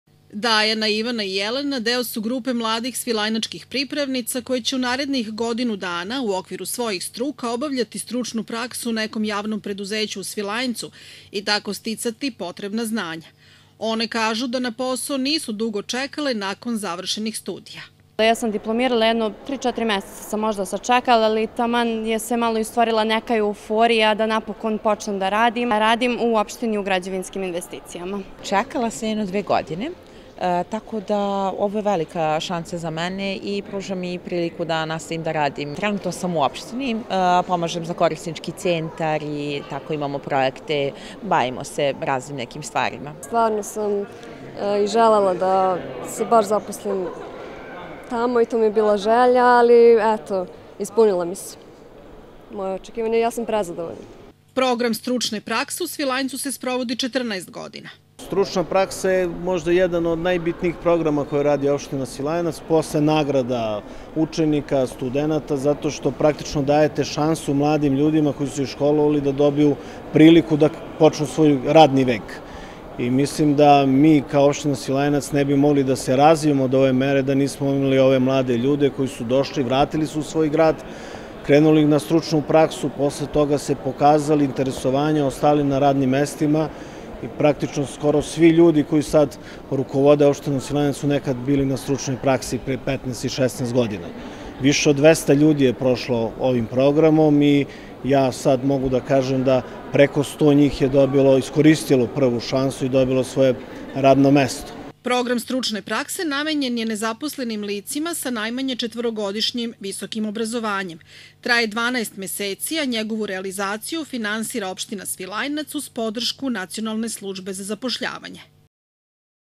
Info - Ugovori za 16 mladih pripravnika u Svilajncu (TV KCN 09.05.2024)
U Prirodnjačkom centru u Svilajncu podeljeni su ugovori za 16 mladih pripravnika koji će svoje prvo zaposlenje dobiti u nekoj od opštinskih javnih ustanova.